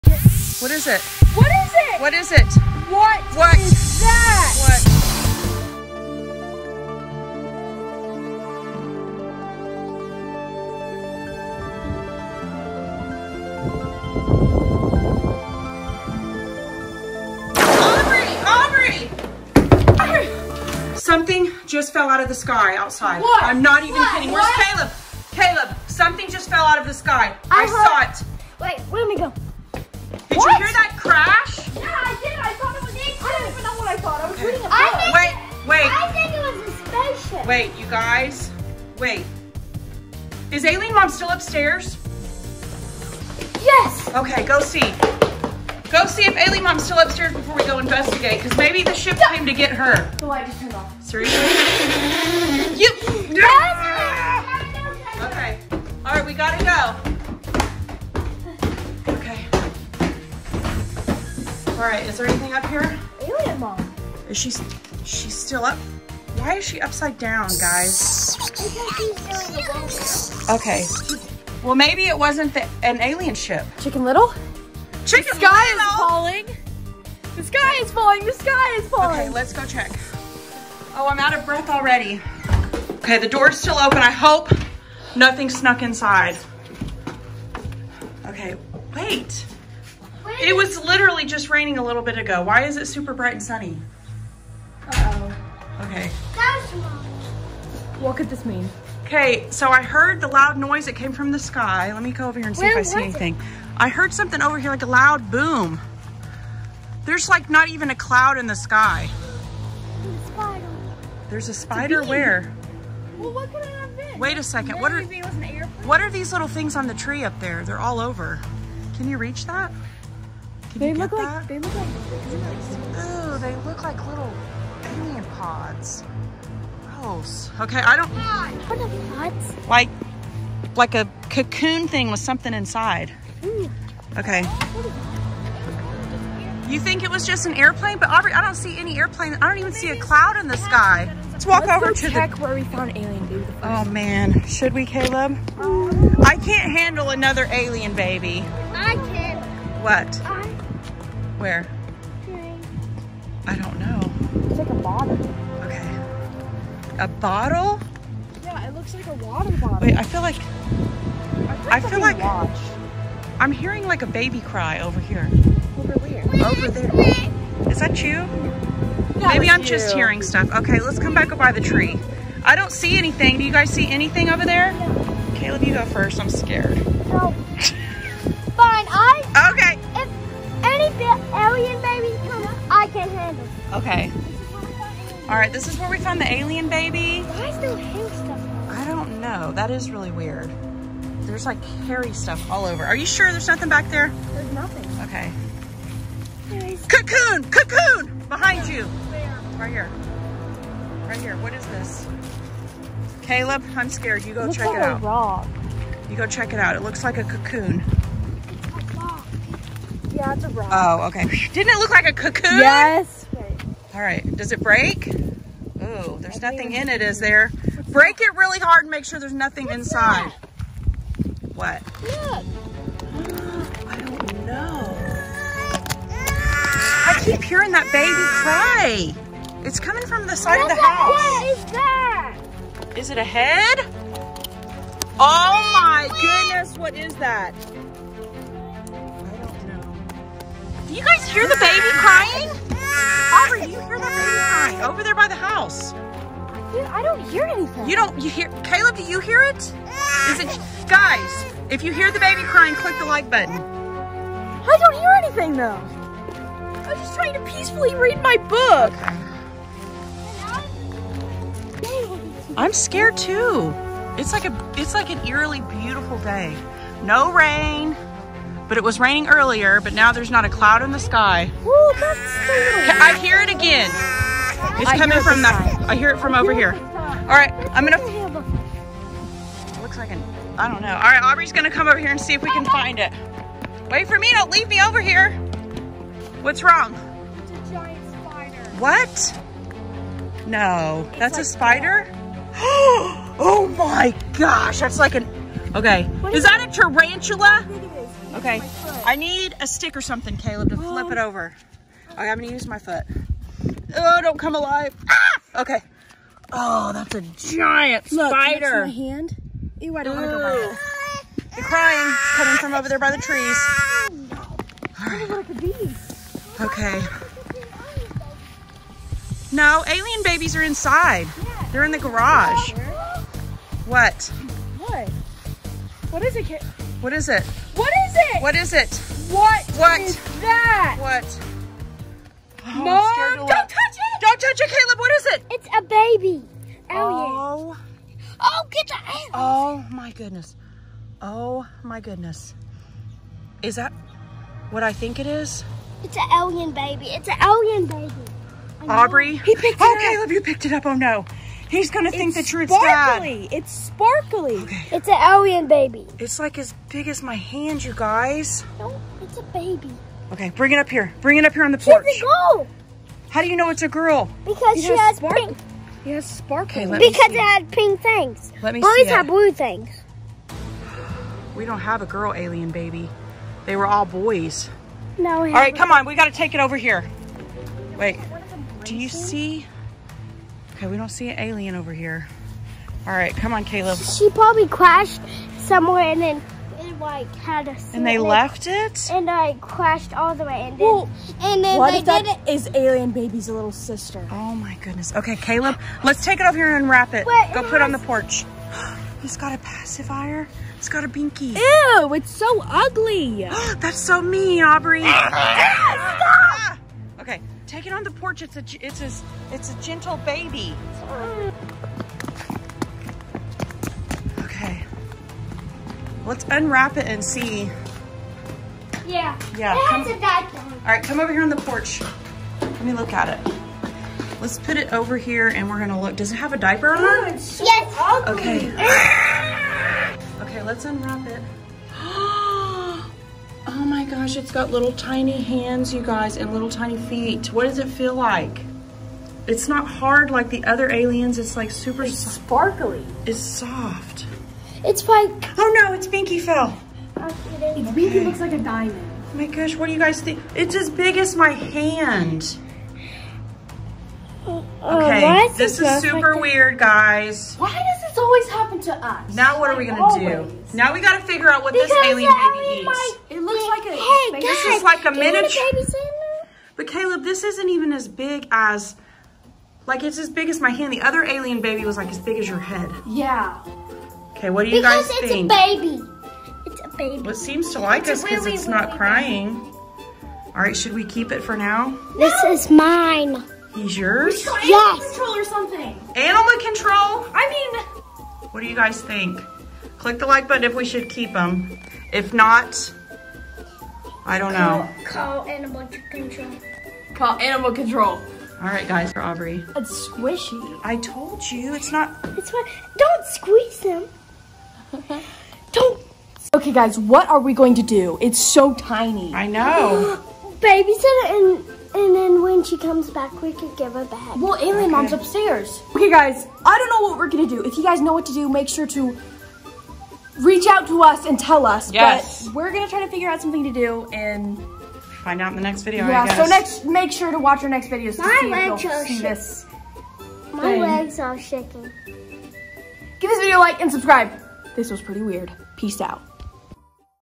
TiNY ALIEN BaBY in OUR YARD! Is it a new Baby Alien?! Aubrey and Caleb Search for Aliens 👽
WE found a TiNY ALIEN Baby in OUR YARD! IS it another BABY ALIEN? Aubrey and Caleb Search for Aliens 👽

Something strange is happening! Mom hears a loud noise and runs to get aubrey and caleb to investigate. Caleb said it sounded like a space ship was crashing but when we go outside we dont' see anything! We look all around and even check to see if alien mom is still up in the attic locked away. SHE is. After awhile we start to hear a baby cry and discover a tiny alien that is a baby alien. SHE is a girl alien. It's so crazy!! What do we do?